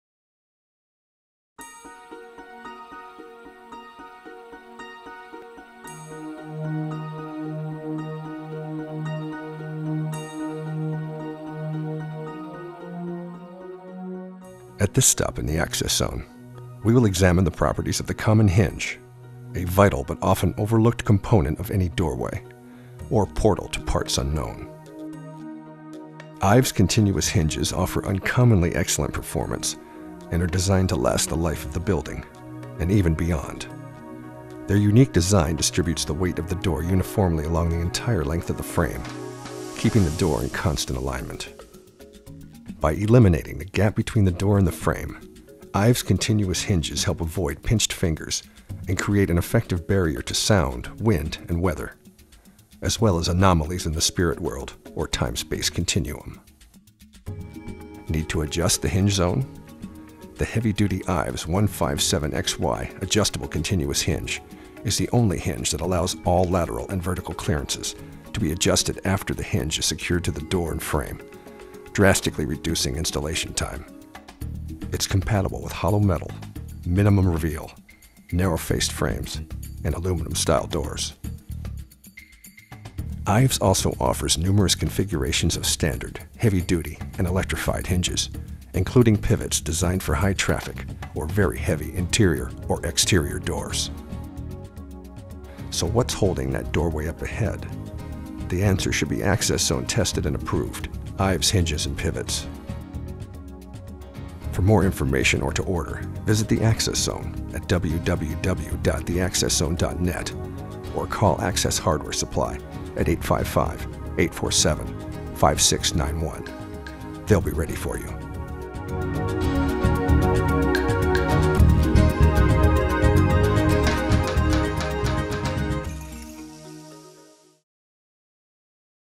Ives Continuous & Adjustable Hinges: What's Holding That Door?
A short overview of Ives Continuous Hinges and their 157XY Adjustable Continuous Hinge, and how their unique design distributes the weight of the door uniformly along the entire length of the frame – keeping the door in constant alignment.